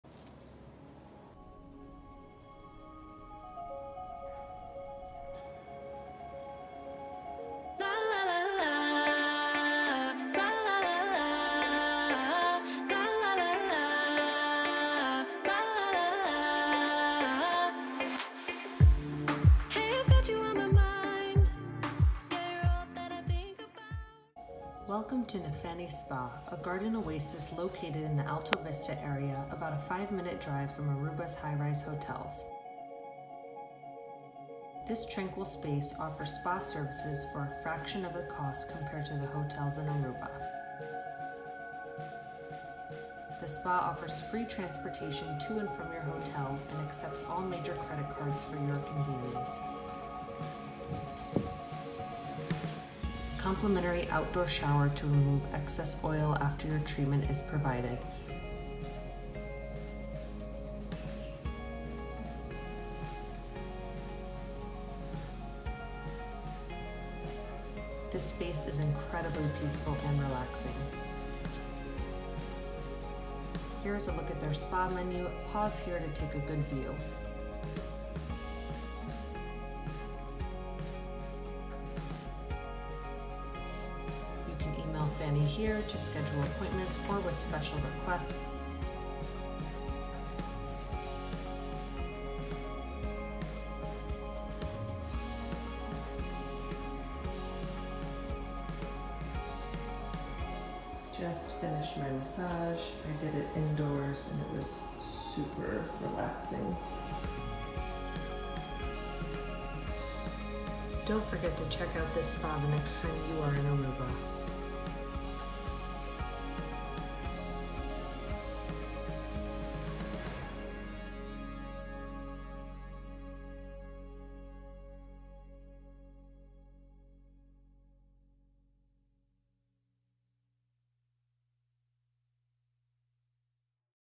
Spa in Aruba that won’t break the bank. Free transportation to and from hotels!
Want spa servies on your next trip to Aruba but don't want to pay those outlandish hotel spa prices? Here is your solution!